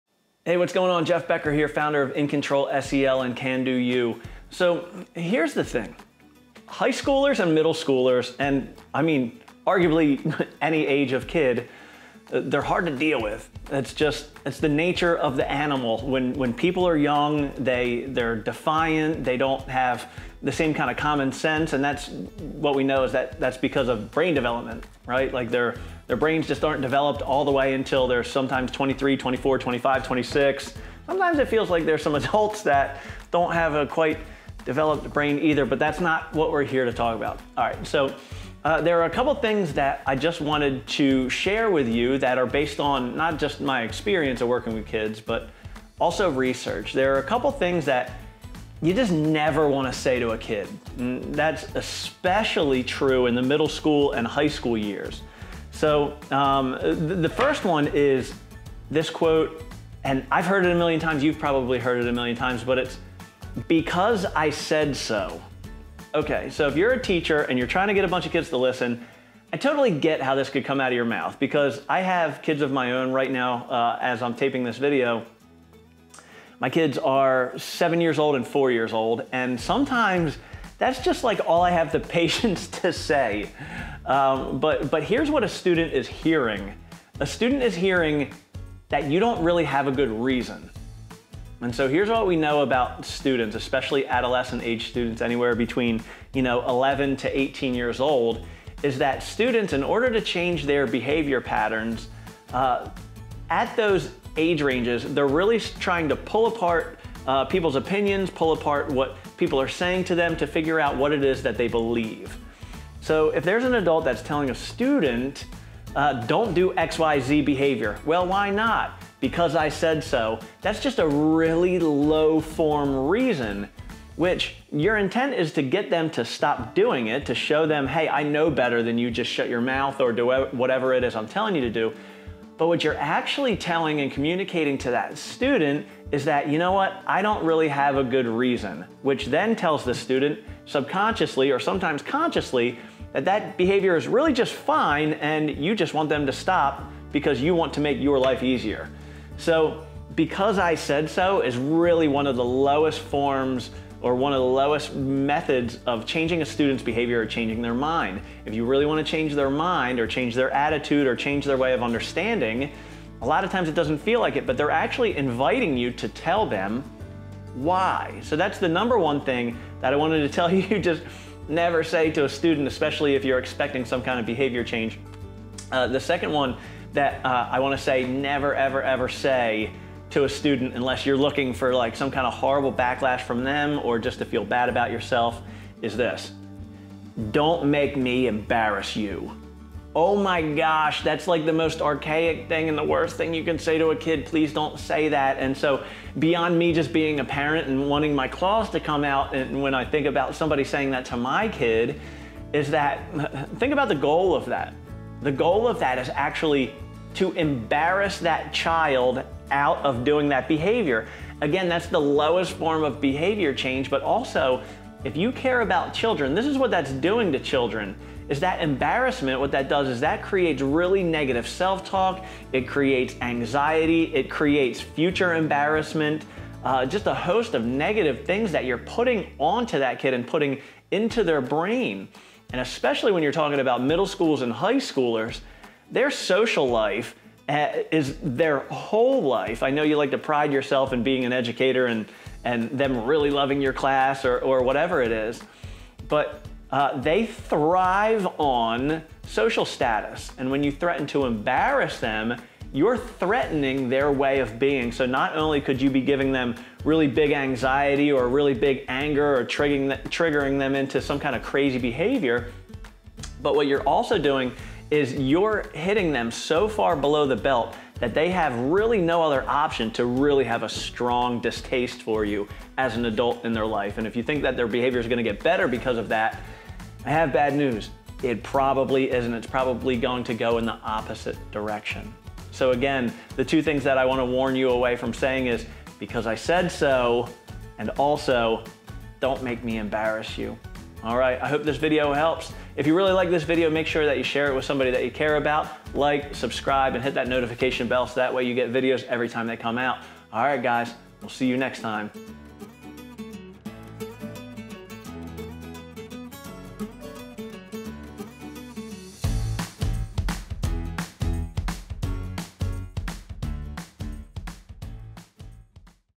NEVER SAY THESE THINGS
If we really want to change the way our students think and behave, there are certain phrases we need to let go of...